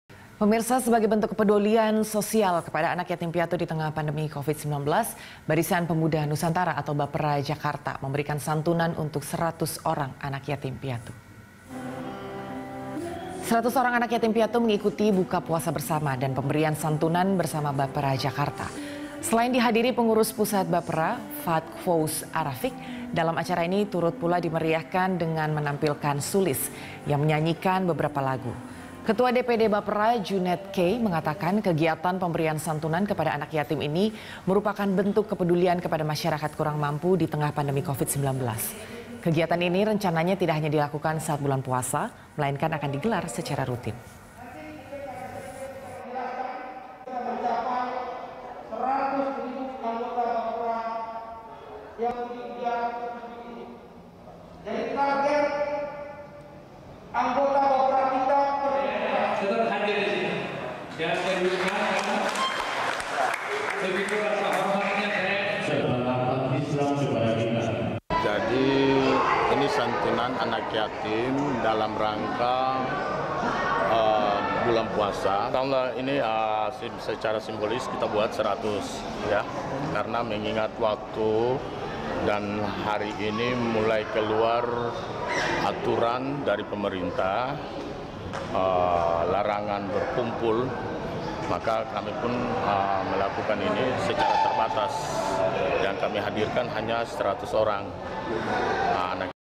Barisan Pemuda Nusantara Bagikan Santunan ke Anak Yatim Piatu
Sebagai bentuk kepedulian sosial kepada anak yatim piatu di tengah pandemi Covid-19, Barisan Pemuda Nusantara (Bapera) Jakarta memberikan santunan untuk 100 orang anak yatim piatu.